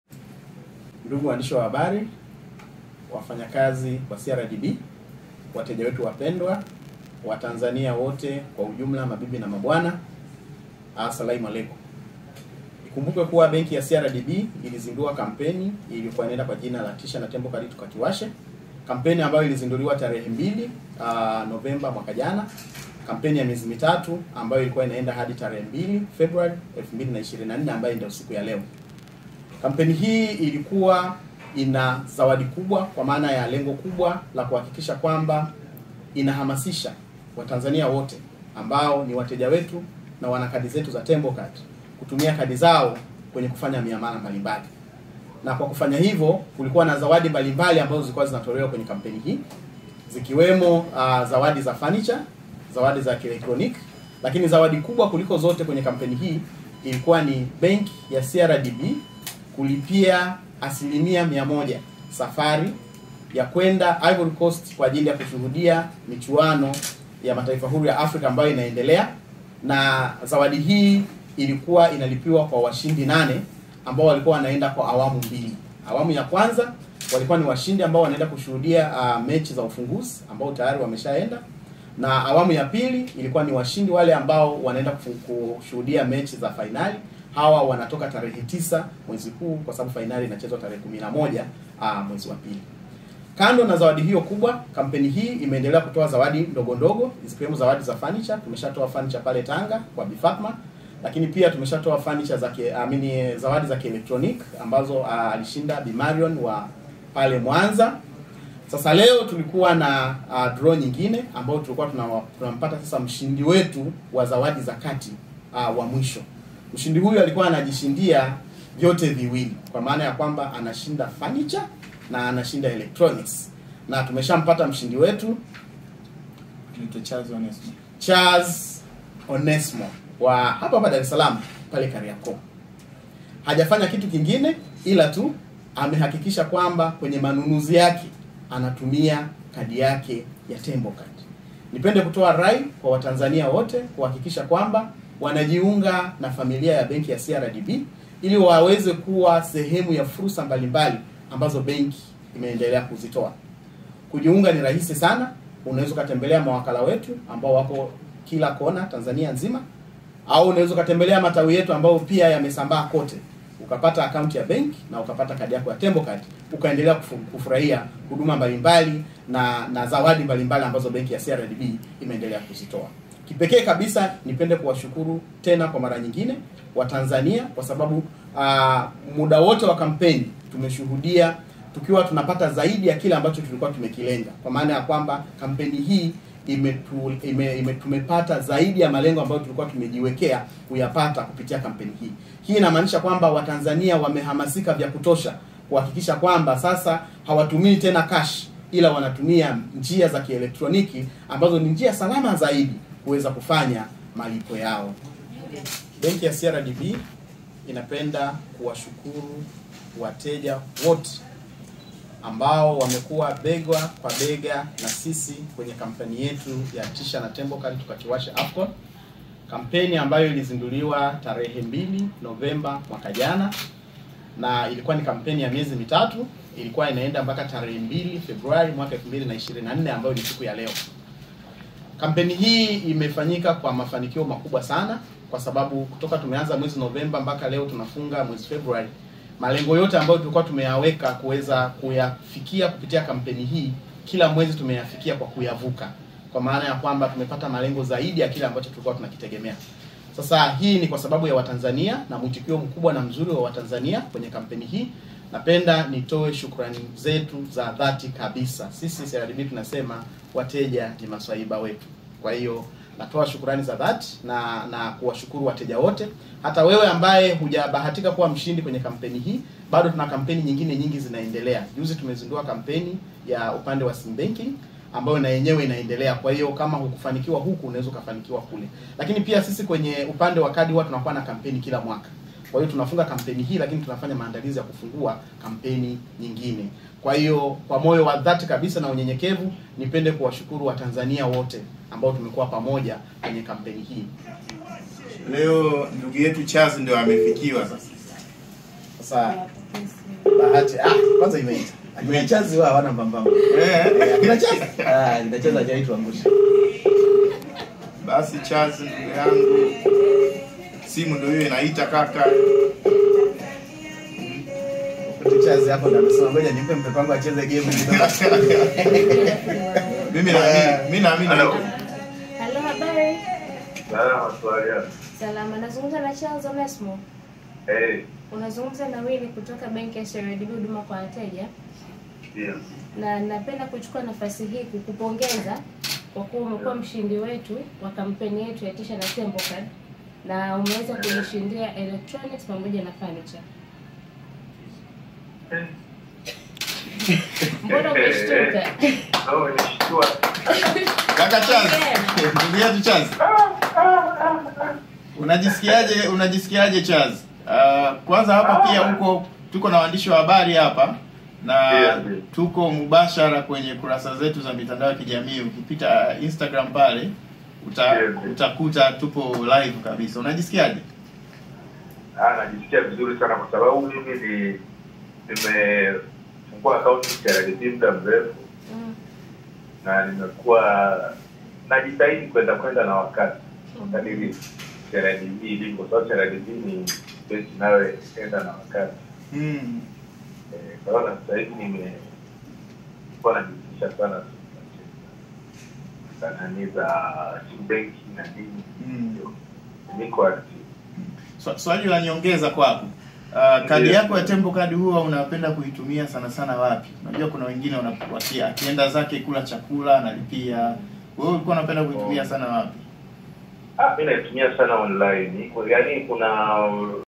HII NOMA BAADA YA WASHINDI WA AFCON,CHARLSE AIBUKA MSHINDI WA MWISHO KWENYE KAMPENI YA TISHA
Sofa seti na vifaa vya kielectronic ni mali halali ya bwa charlse onesmo ambae ameibuka mshindi katika draw ya mwisho wa ya kampeni ya tisha